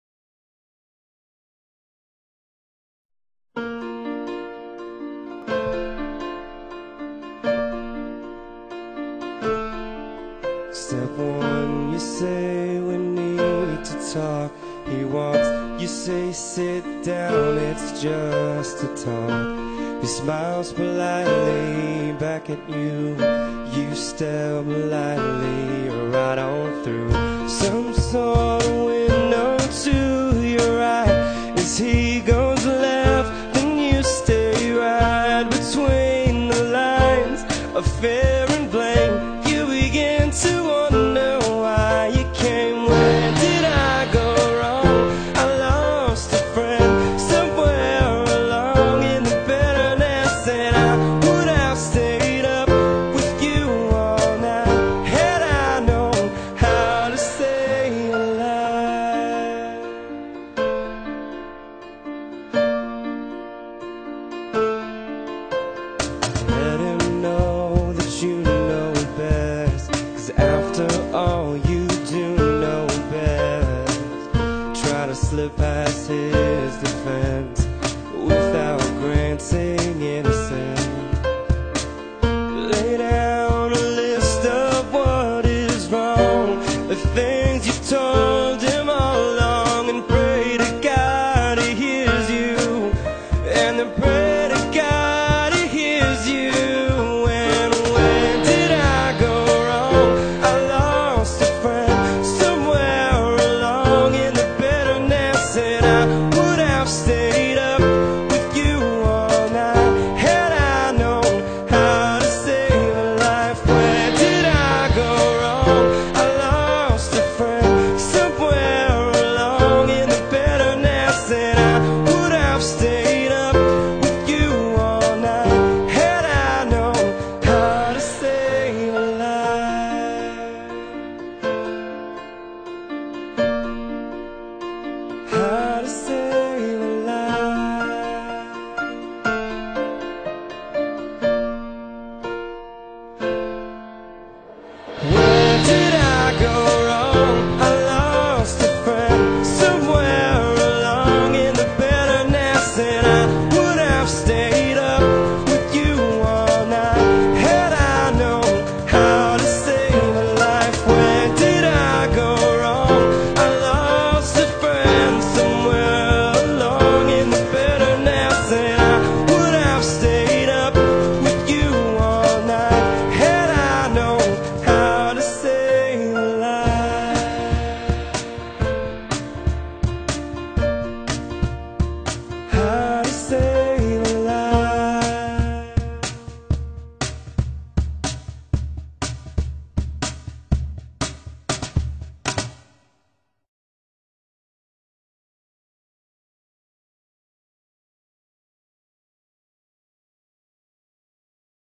Departed Batchmates Juan R. Liwag Batch 1993
high school photos by batchmates video tribute Juan R. Liwag Memorial High School Batch 1993